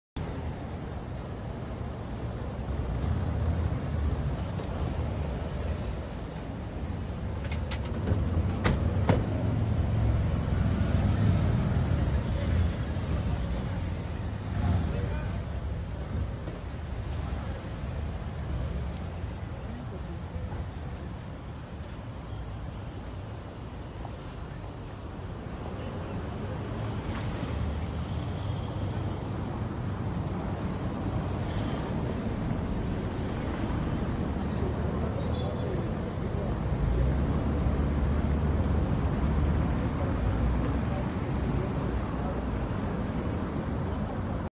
Arriba a Casa Aguayo, Ana Lucia Hill, luego de ser nombrada encargada de despacho
En estos momentos arriba a Casa Aguayo, Ana Lucia Hill
, luego de ser nombrada encargada de despacho del gobierno de Puebla